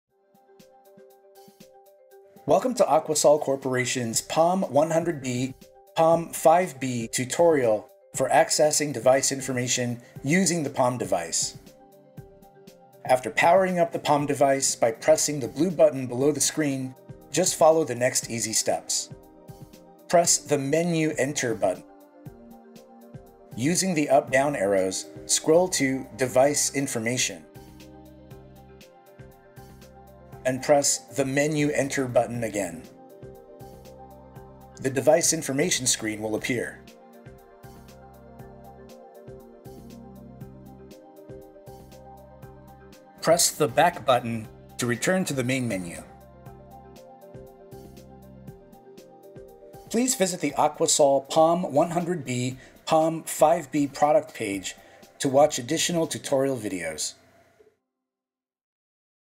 POM-100B/POM-5B: Accessing Device Information
Watch instructions on how to access the device information to retrieve the POM-100B/POM-5B model, serial number and calibration information.